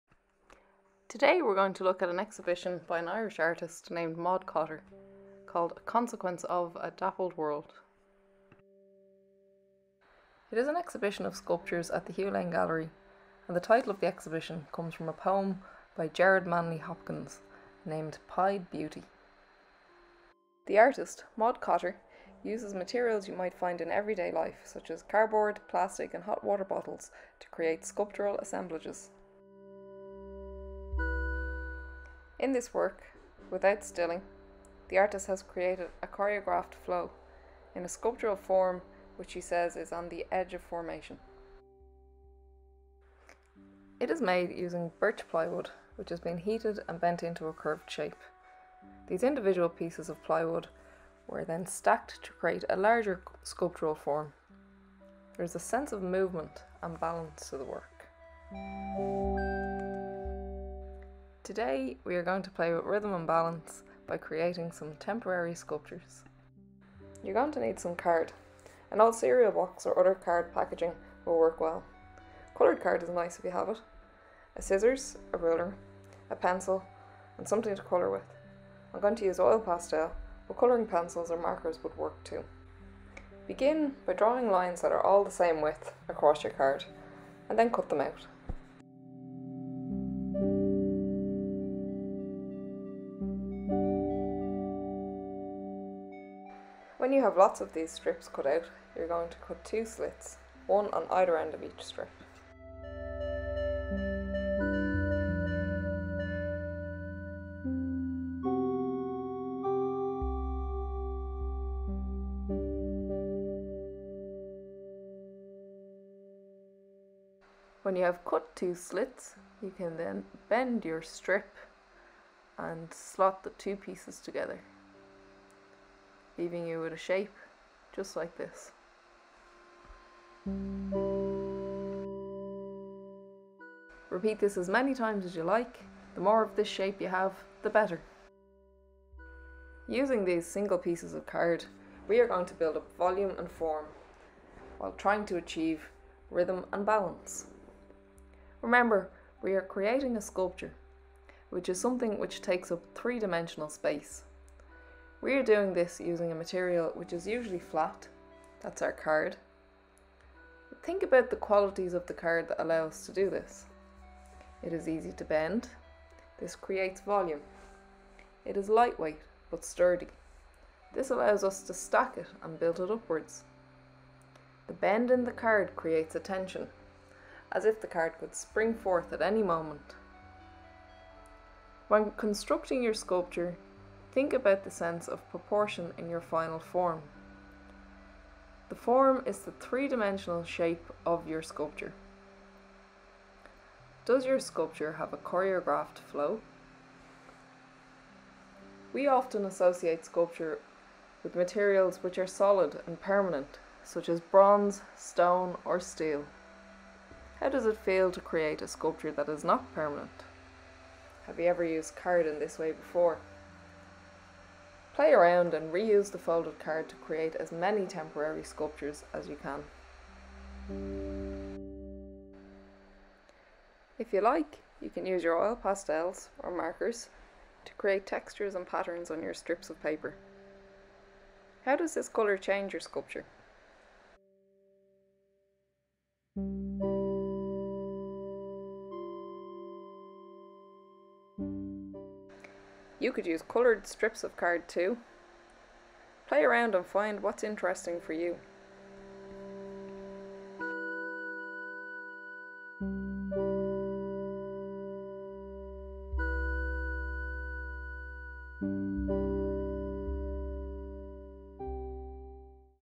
Rhythm & Balance
In this film artist Jane Fogarty looks at the work of Maud Cotter currently on view at the Hugh Lane Gallery in the exhibition a consequence of - a dappled world. Jane invites you to explore the themes of rhythm and balance when making your own temporary 3D artworks.